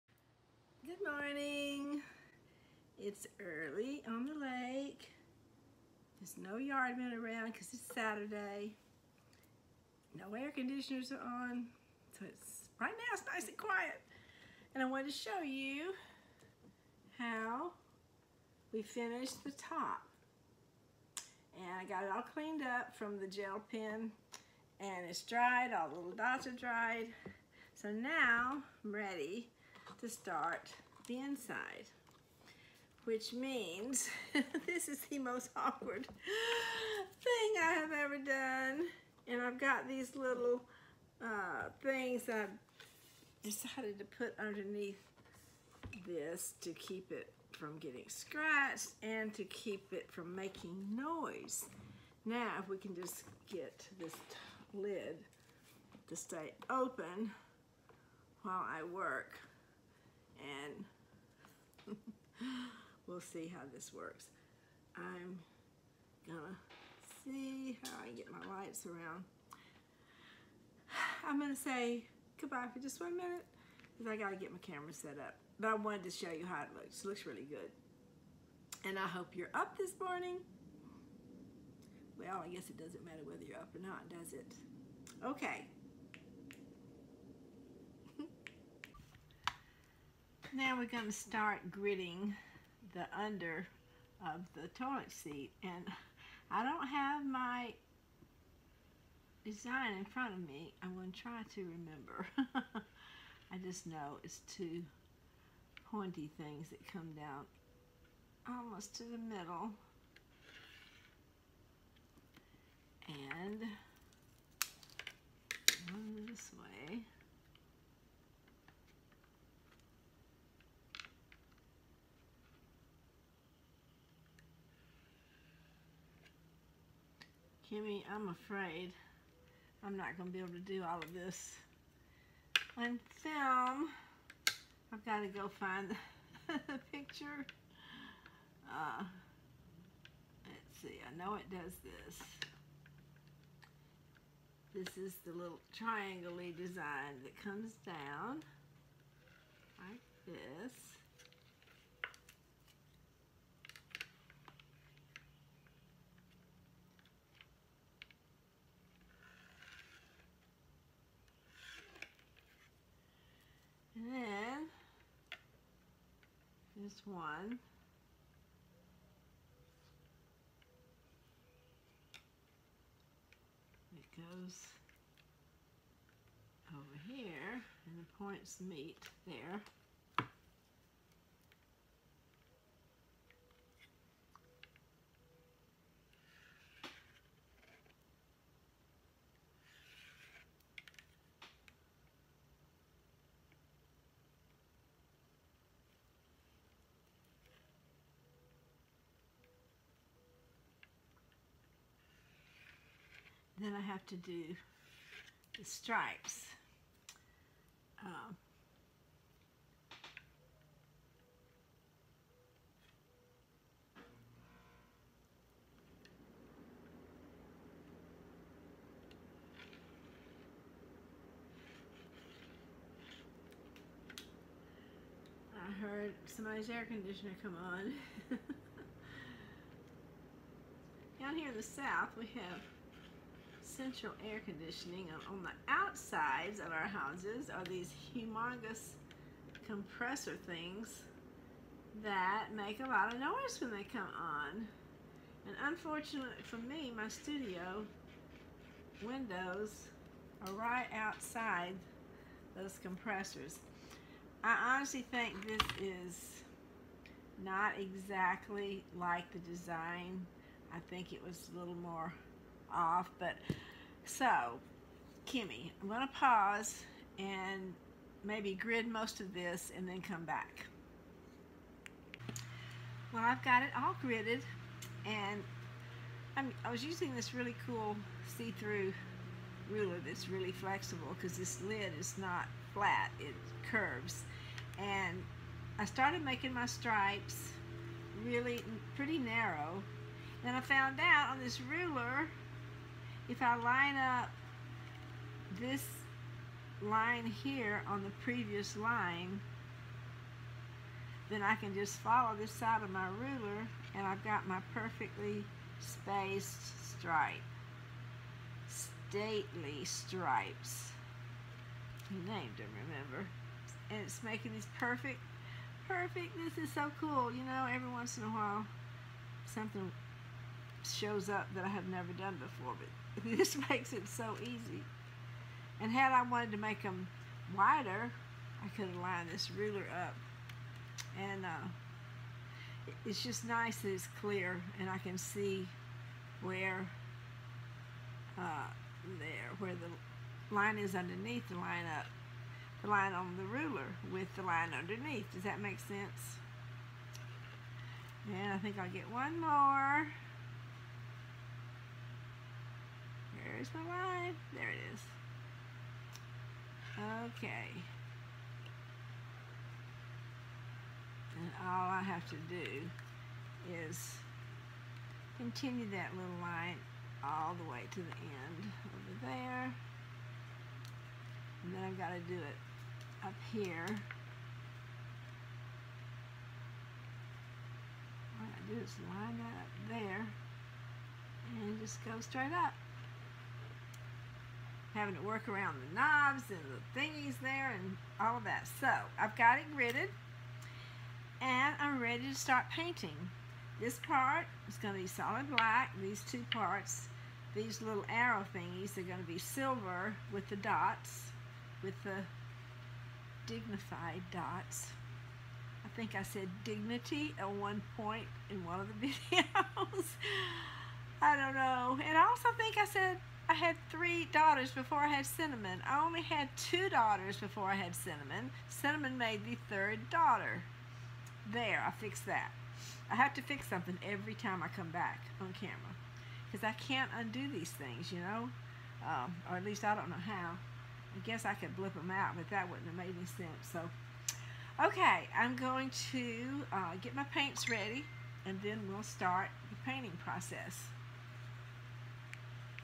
The Toilet Seat Project - Part 6 - Gridding Seat Back
This video is about The Toilet Seat Project - Part 6 - Gridding Seat Back.   A toilet seat painted for Kimmy Schwimmy in The Finisher (guitar) design.  This is a 'how to' from start to finish of how Kayren Spain of KAYREN SPAIN Designs painted this toilet seat.